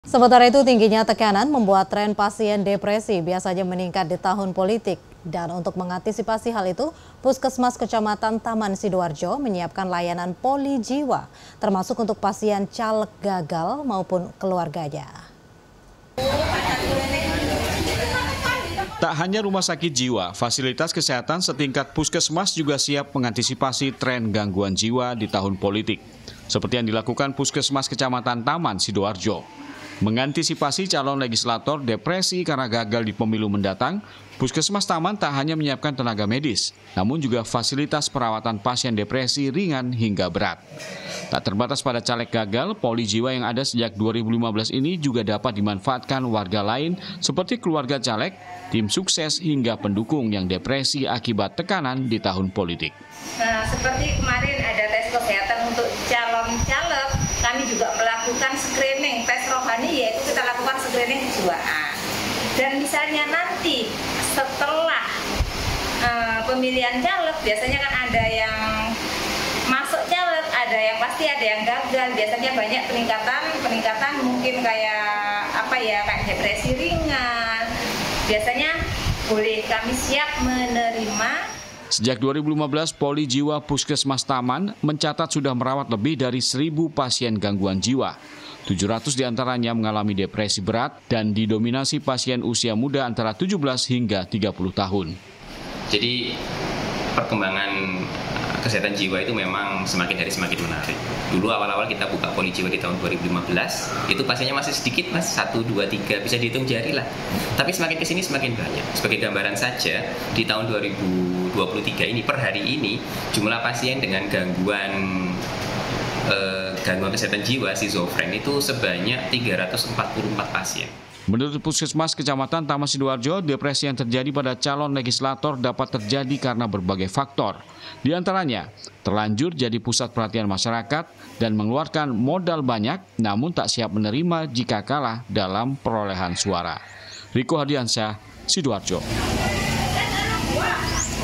Puskesmas Siap Terima Pasien Caleg Depresi
Tingginya Tekanan Membuat Tren Pasien Depresi Biasanya Meningkat di Tahun Politik. Untuk  Mengantisipasi Hal Itu, Puskesmas Kecamatan Taman Sidoarjo Menyiapkan Layanan Poli Jiwa, Termasuk Untuk Pasien Caleg Gagal Maupun Keluarganya.